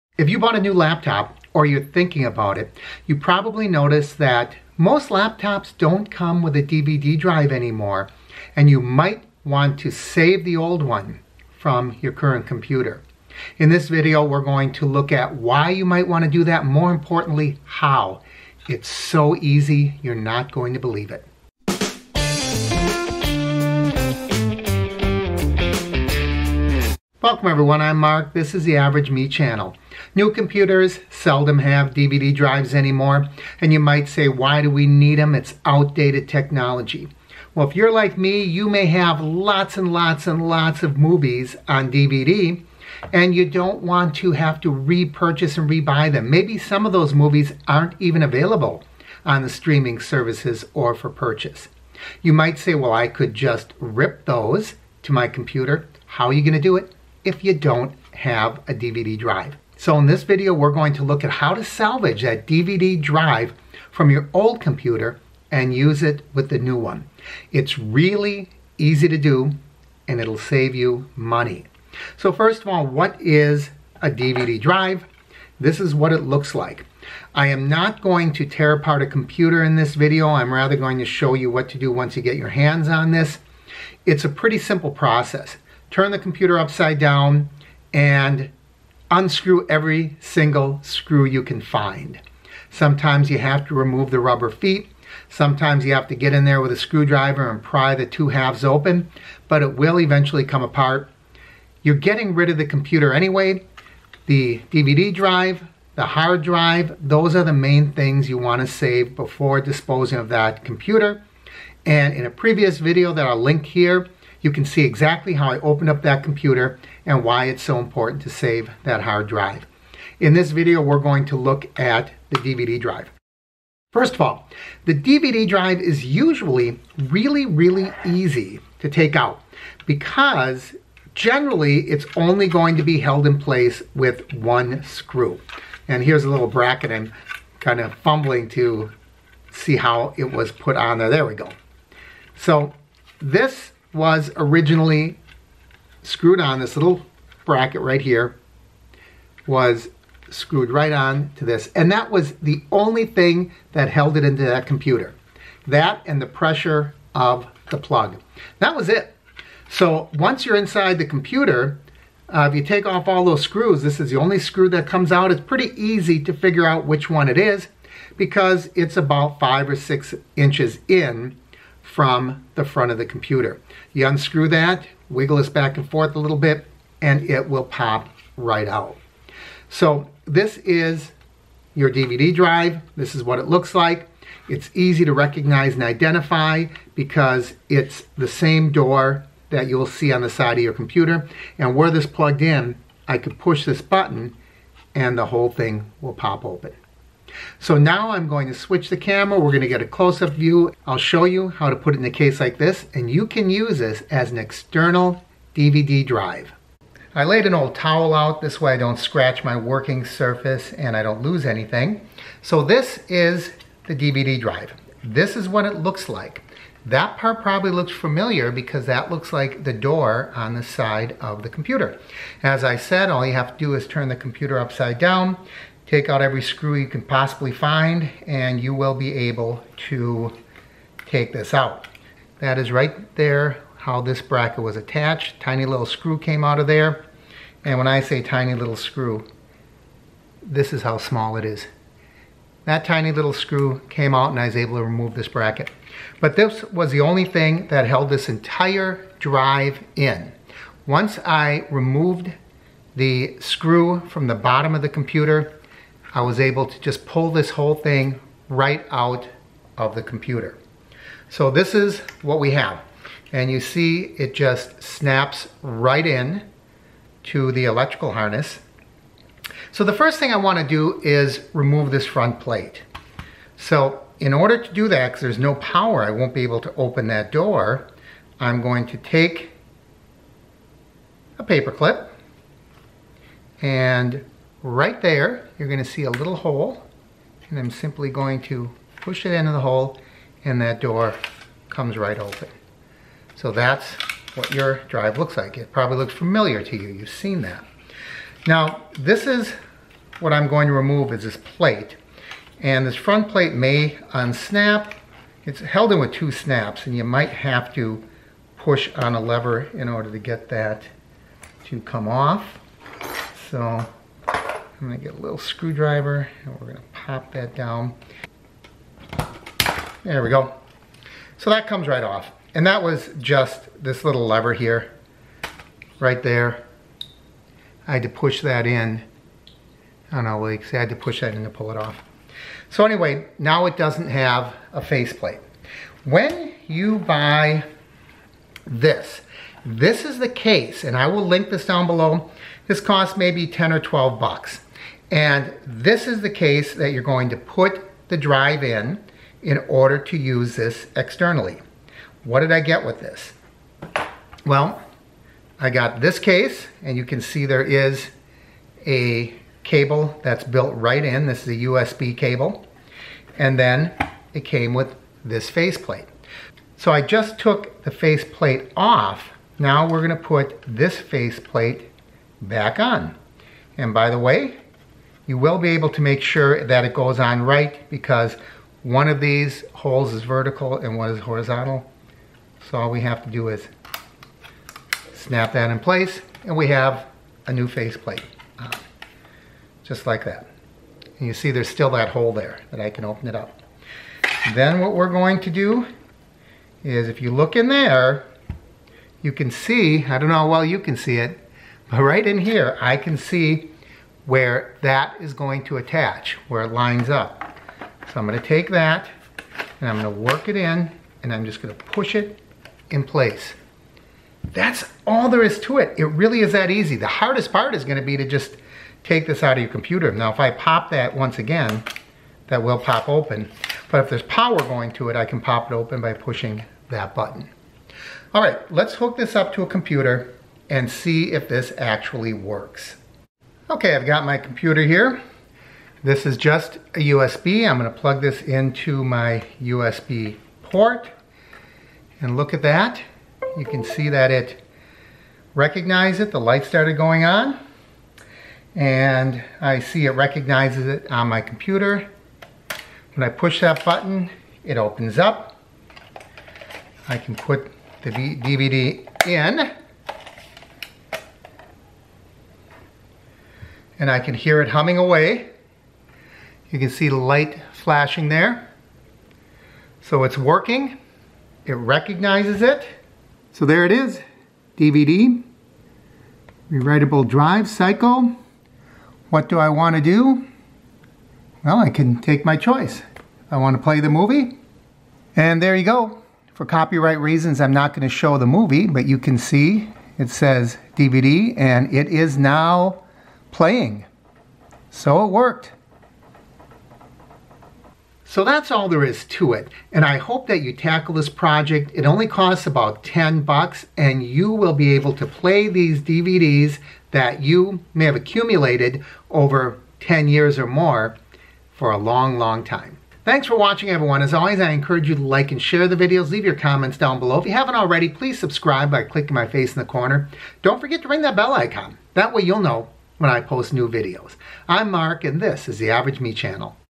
Turn Your Old Laptop DVD Drive into an External Drive! Easy to do!
DVD case I used
The camera used in this video was a Canon G7X Mark II

Don't throw away all your DVDs! You can still play them on your laptop! Do you have an old laptop with a DVD drive? This video will show you how to re-use that DVD drive so you'll be able to play cd's, DVDs, and movies. Most new laptops don't have a DVD drive, and saving the old one is so easy!